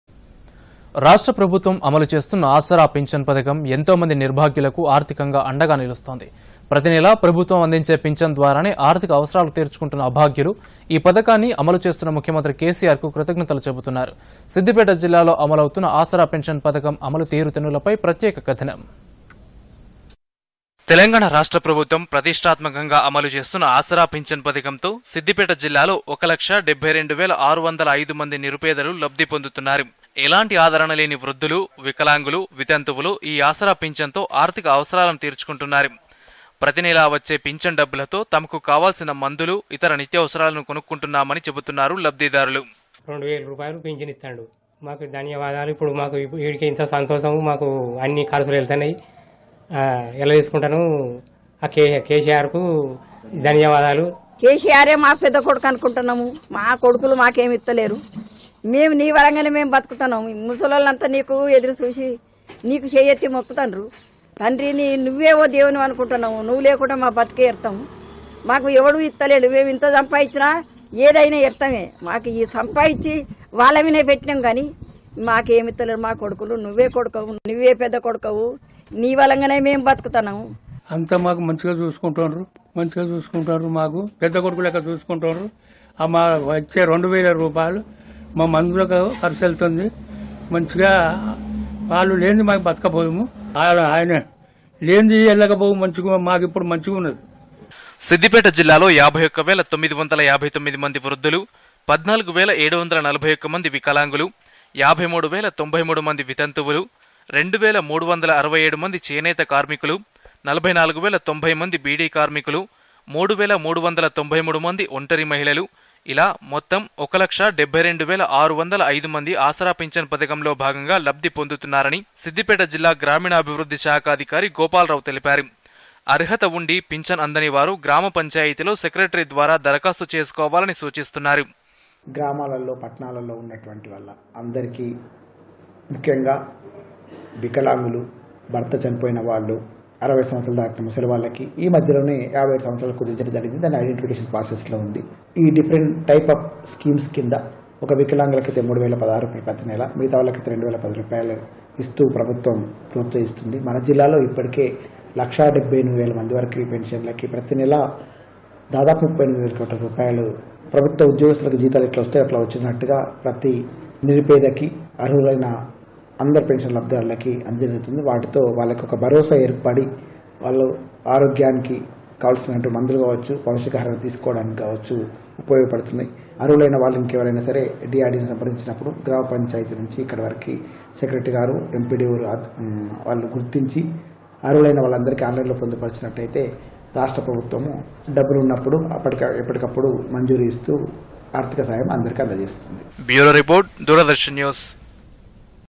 Siddipet : Asara pension scheme Special Story 🟥 DD News Telangana
రాష్ట్ర ప్రభుత్వం అమలు చేస్తున్న ఆసరా పించన్ పథకం.. ఎంతోమంది నిర్భాగ్యులకు ఆర్థికంగా అండగా నిలుస్తోంది. ప్రతినెలా ప్రభుత్వం అందించే పించన్ ద్వారానే ఆర్థిక అవసరాలు తీర్చుకుంటున్న అభాగ్యులు.. ఈ పథకాన్ని అమలు చేస్తున్న ముఖ్యమంత్రి కేసీఆర్ కు కృతజతలు చెబుతున్నారు. సిద్దిపేట జిల్లాలో అమలవుతు16:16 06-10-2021న్న ఆసరా పెన్షన్ పథకం అమలు తీరుతెన్నులపై ప్రత్యేక కథనం.